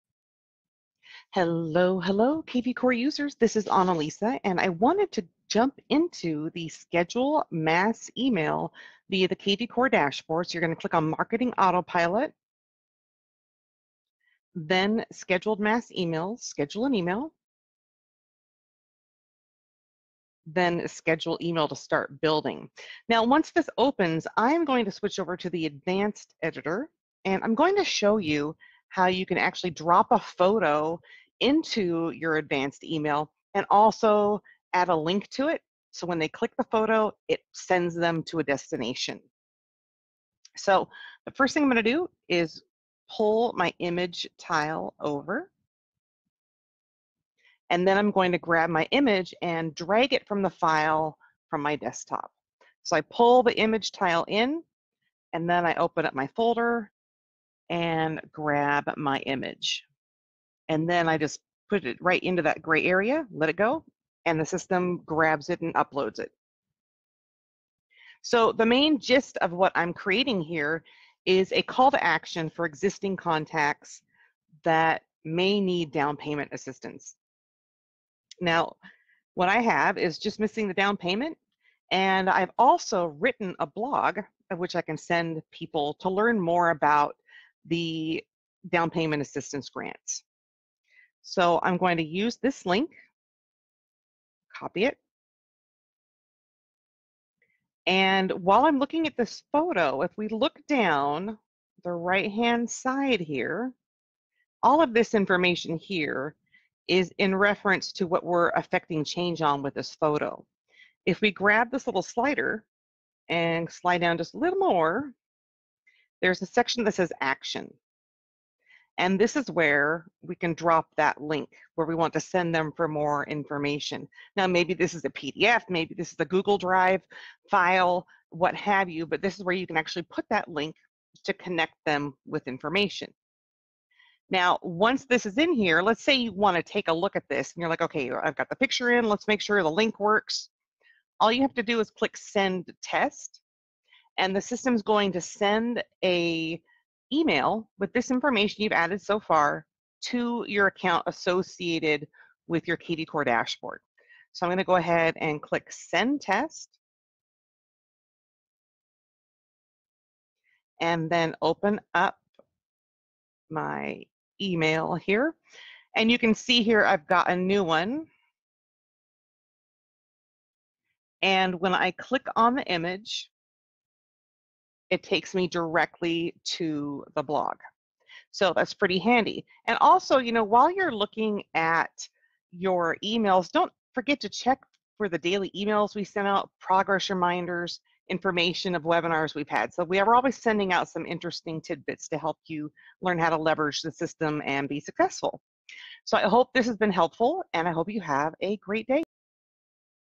kvCORE Adding a Clickable Image into the Advanced Email Editor
Learn How to Add a Clickable Image into the Advanced Email Editor within kvCORE.